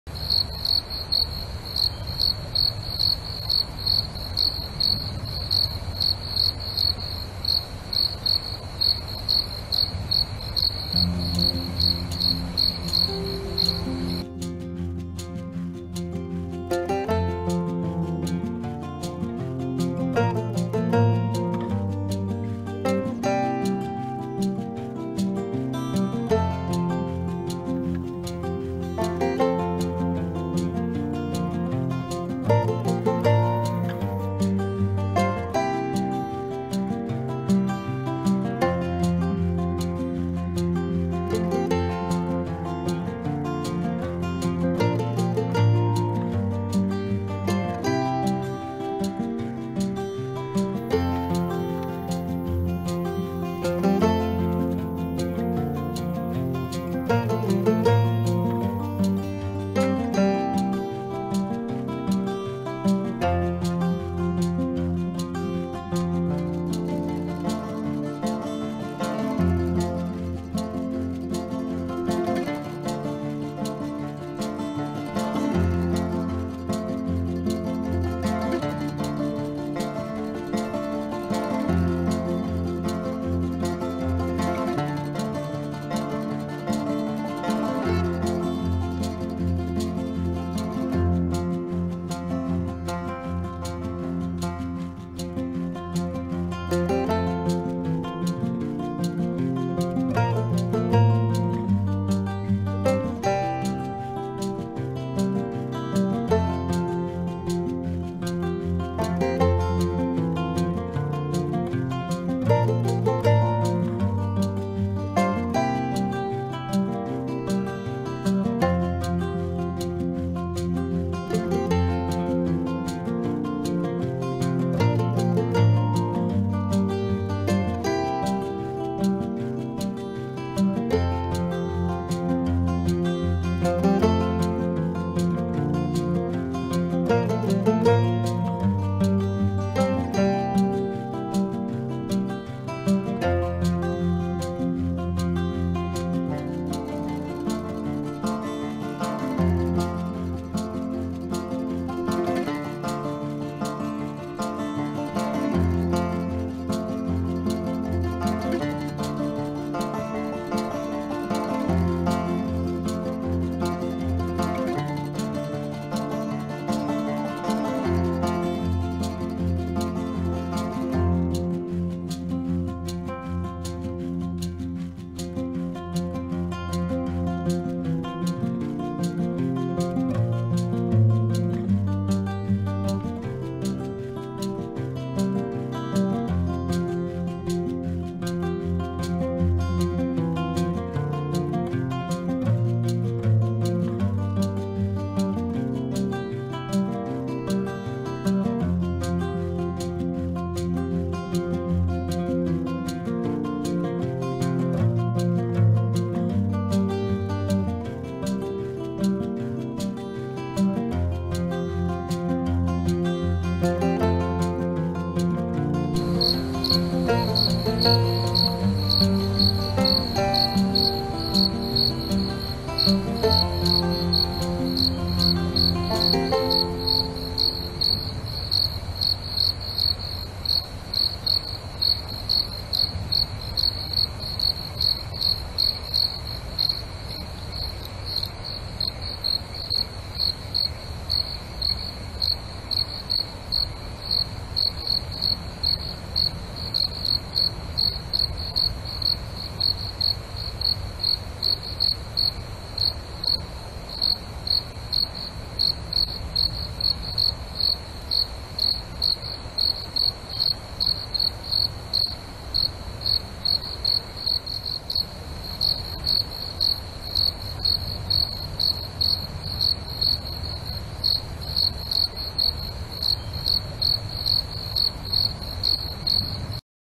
Cute Spider Story รวมภาพแมงมุมน่ารักๆ
รวมภาพน่ารักๆ ของเหล่าแมงมุมที่กำลังเดิน กินอาหาร นอนและทำรัง

ขอบคุณทุกท่านที่ติดตามรับชม และที่ช่วยกดไลค์ กดแชร์  กดติดตาม และกดกระดิ่งติดตาม